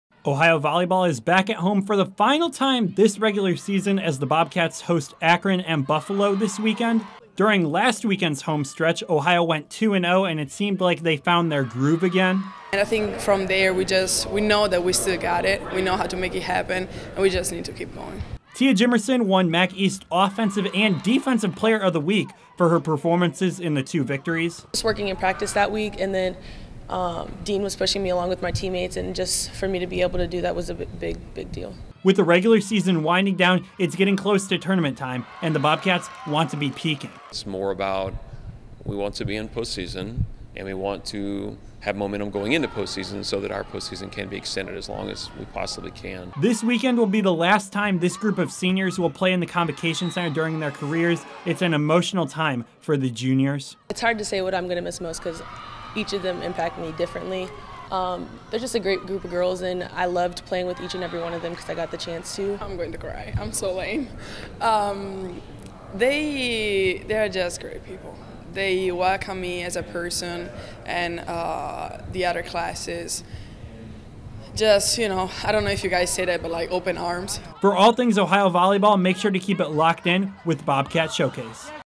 Ohio Volleyball: Bobcats prep for final regular season games.
It's the last regular season weekend for the Ohio volleyball team. Hear from Head Coach Deane Webb and Juniors Tia Jimerson and Vera Giacomazzi.